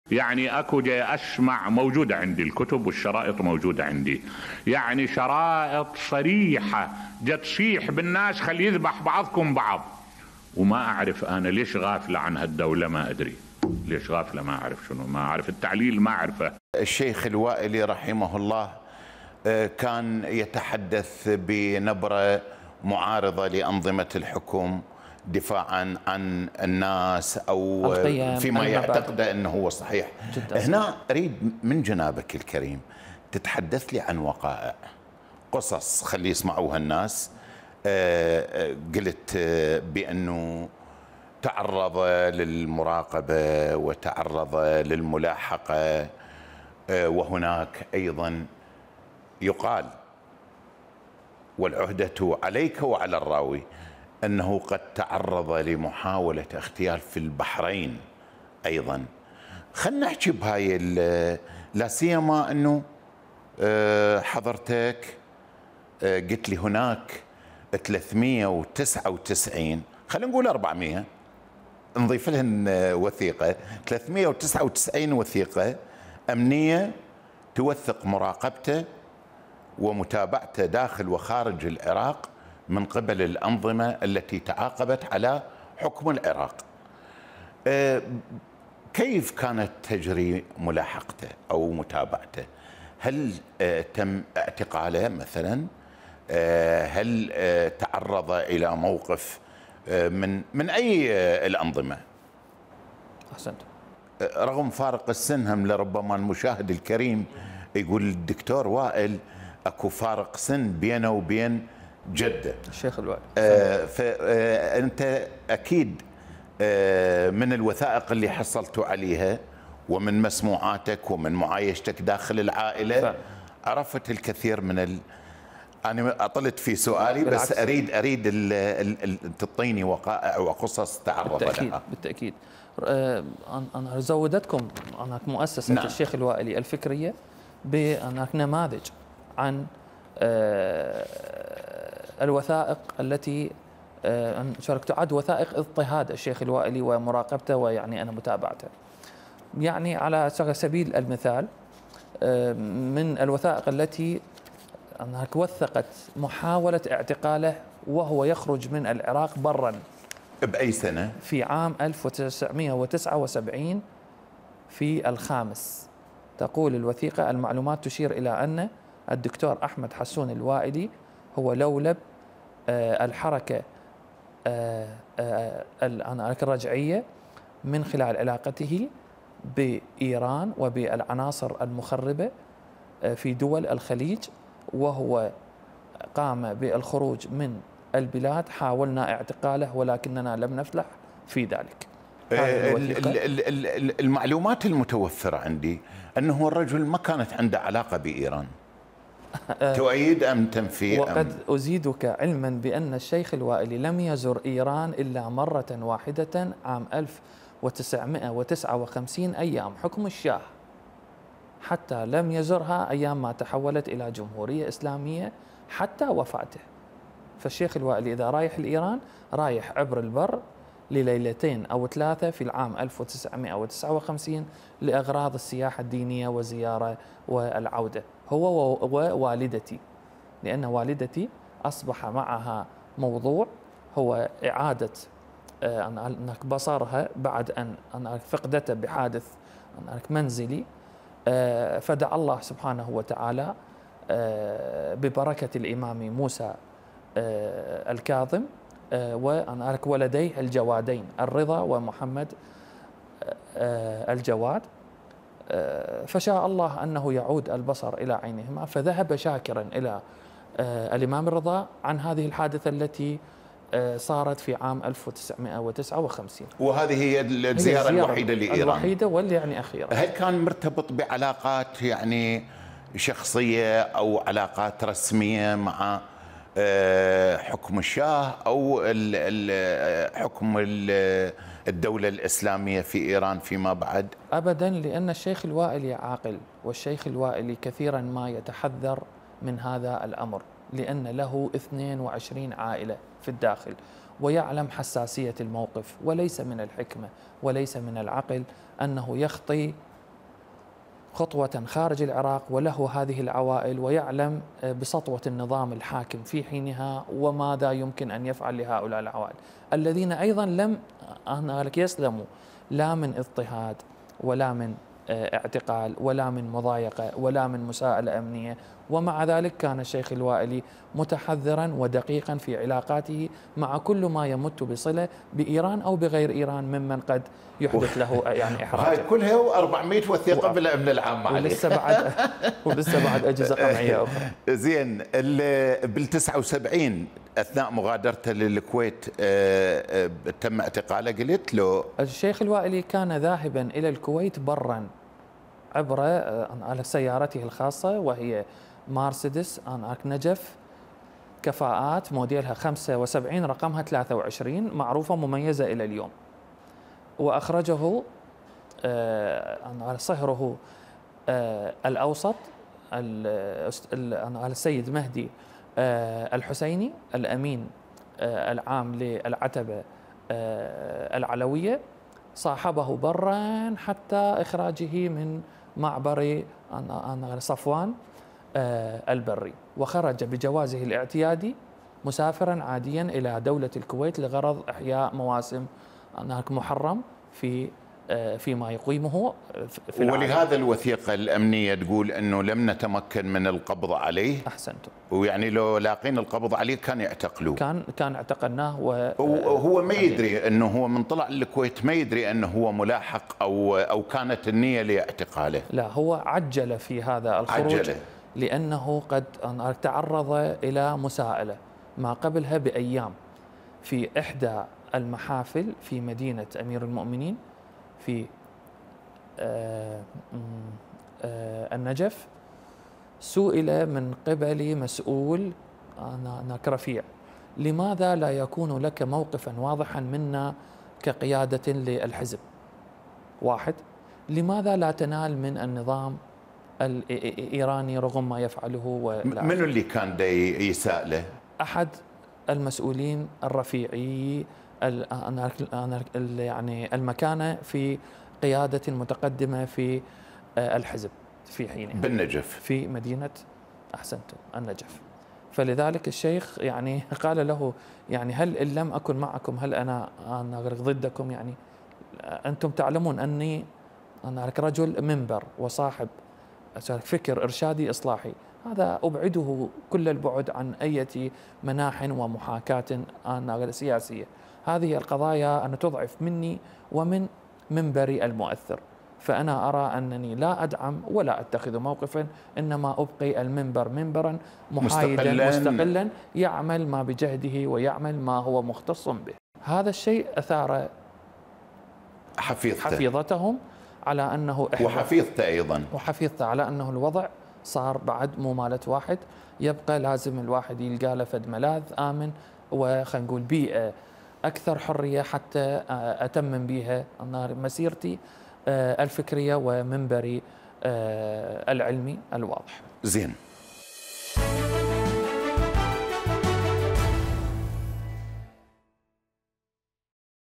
الشيخ احمد الوائلي وقائع وقصص تروى لأول مرة على لسان حفيده
موقع قناة التغيير الفضائية  الالكتروني